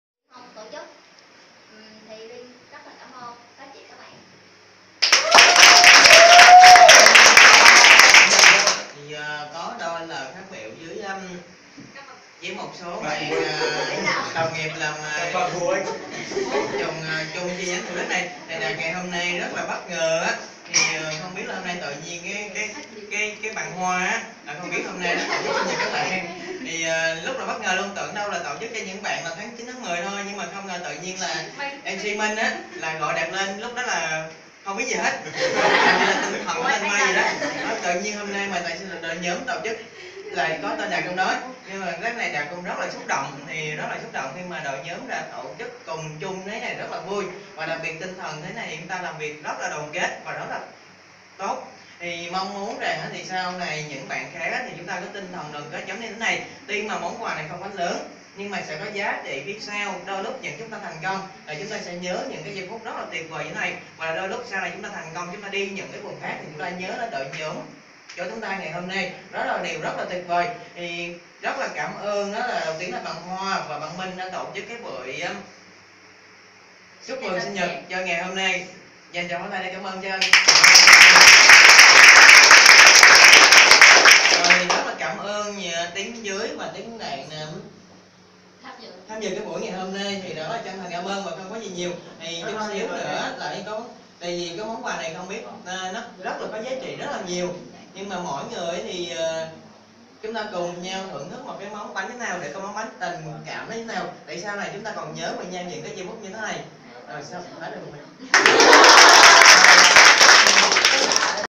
Happ brithday (cty TNHH Tầm nhìn Đại Hưng 668)
Sếp đạt chia sẻ cảm xúc. Quân quay